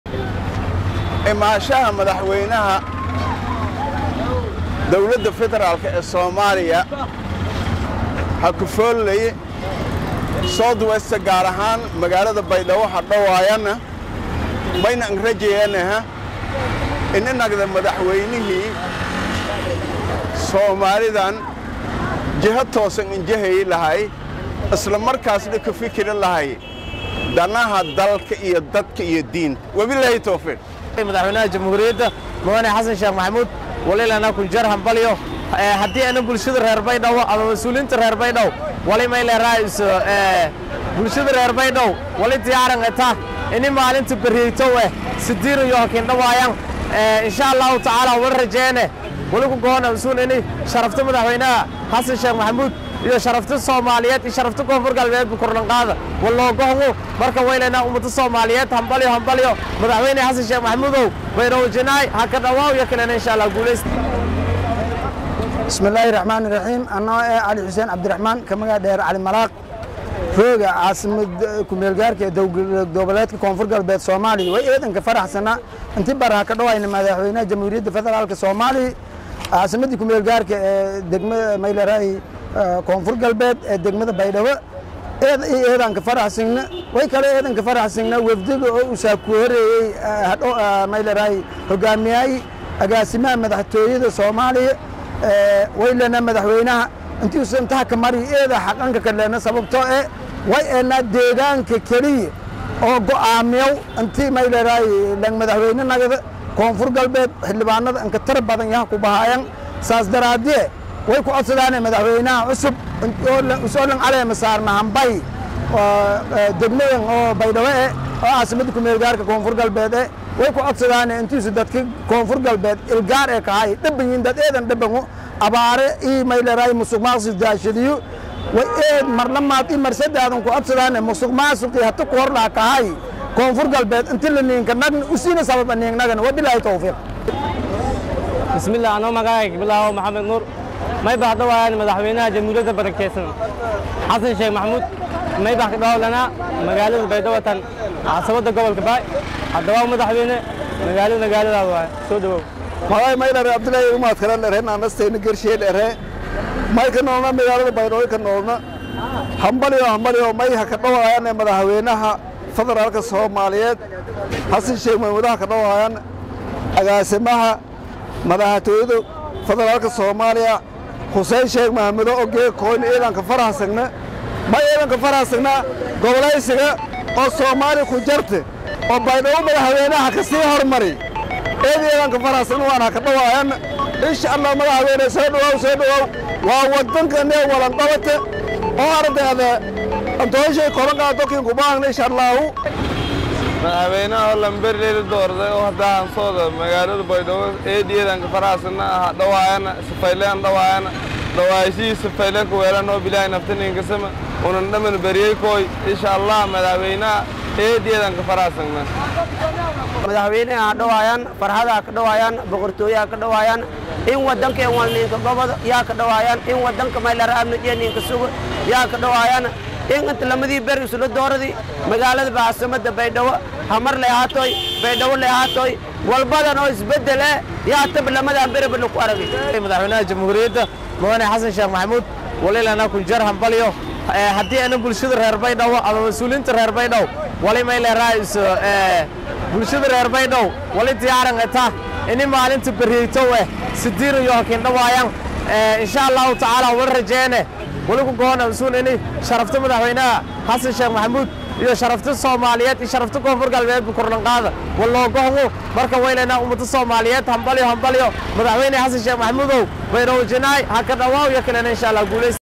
DEG DEG Xogta Safarka Madaxweyne Xasan Sheekh uu ku tegaya Baydhabo
DEG DEG Xogta Safarka Madaxweyne Xasan Sheekh uu ku tegaya Baydhabo iyo shacabka baydhabo oo si weyn uga hadlay